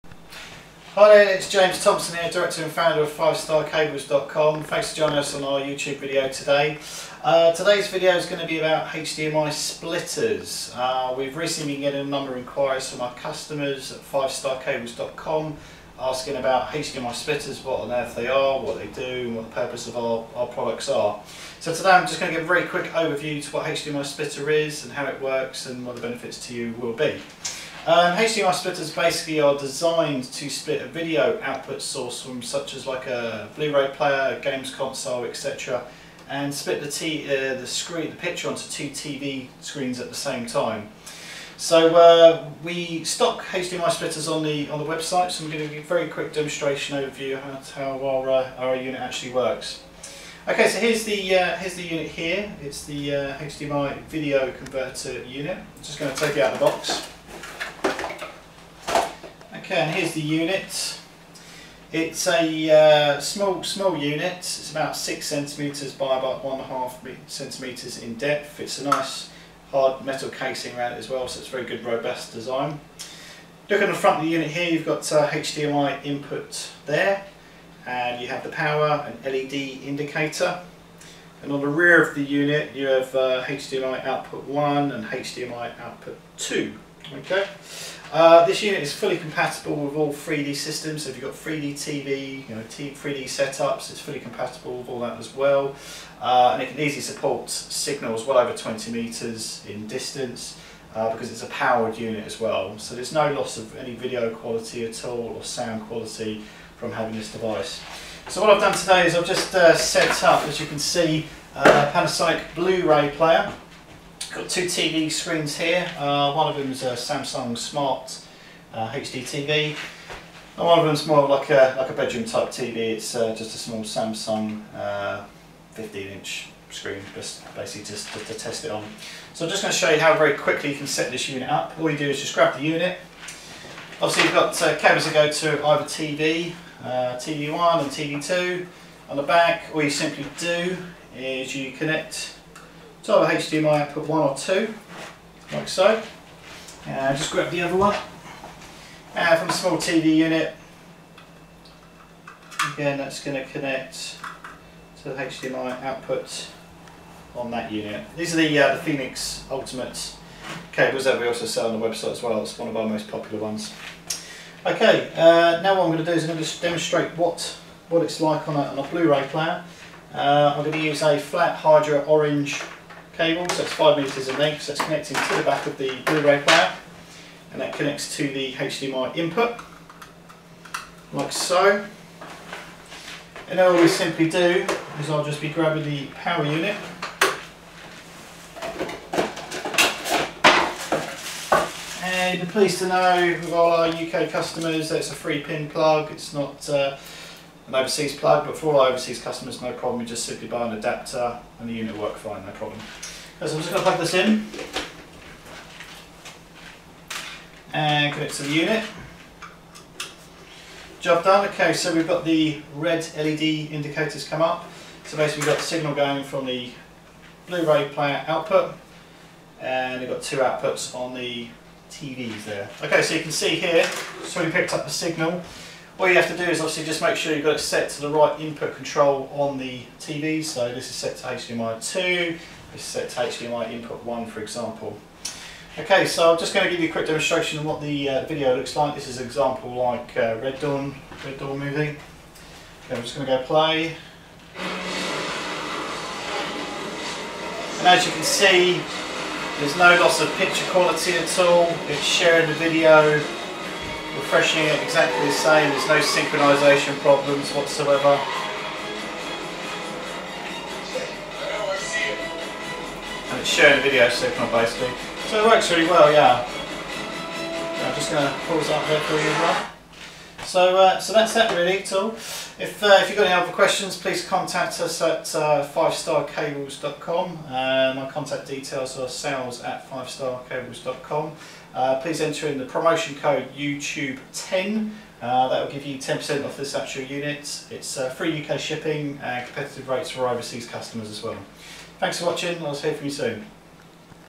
HDMI Splitter - 1 x input to 2 x Output Active Amplifier setup Demonstration
due to popular demand for HDMI Splitters and questions I have uploaded a demonstration Video.

James